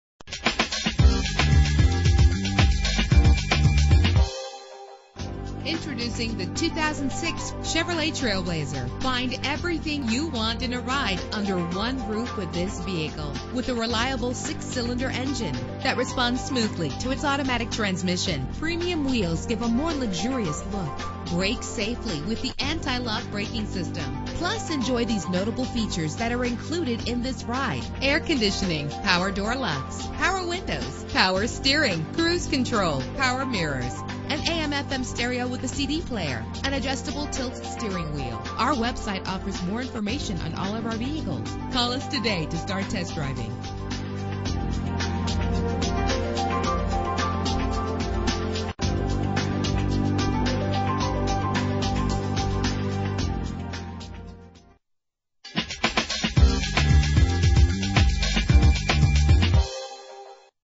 2006 Chevrolet TrailBlazer - Lynnwood WA
Year: 2006
Make: Chevrolet
Model: TrailBlazer
Trim: LS
Engine: 4.2 liter inline 6 cylinder 24 valve
Transmission: 4-Speed Automatic
Color: White
Mileage: 128528
Address:
21301 Hwy